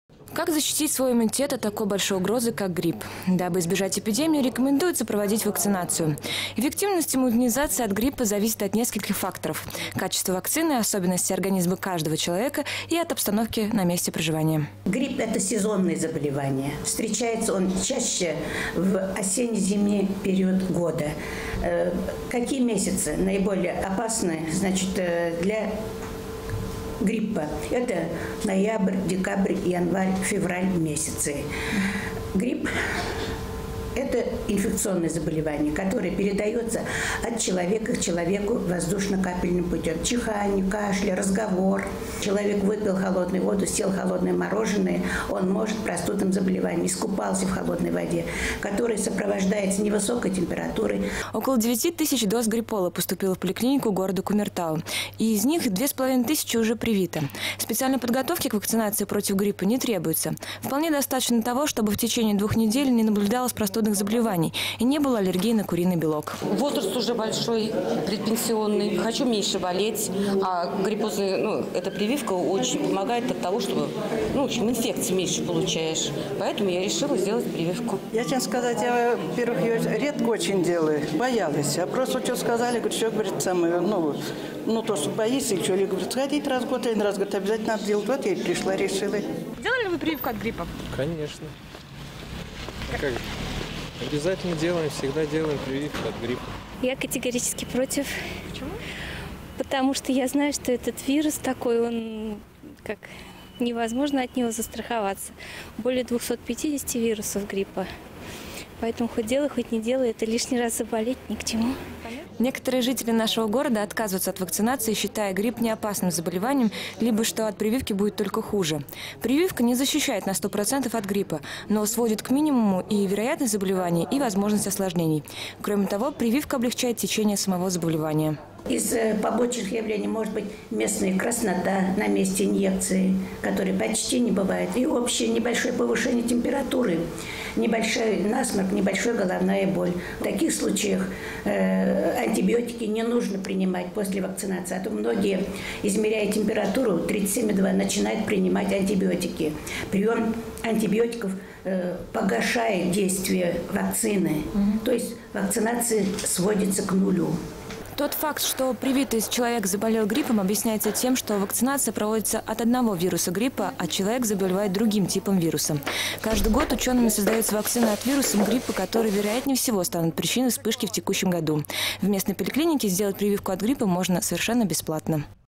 АРИС Вакцинация от гриппа
До конца октября в России планируется завершить плановую вакцинацию от гриппа. Так как вирусы гриппа постоянно меняются, рекомендуется проходить её ежегодно. После прививки защита от гриппа развивается в течение двух недель и действует около года — поэтому делать прививку имеет смысл осенью до наступления холодов.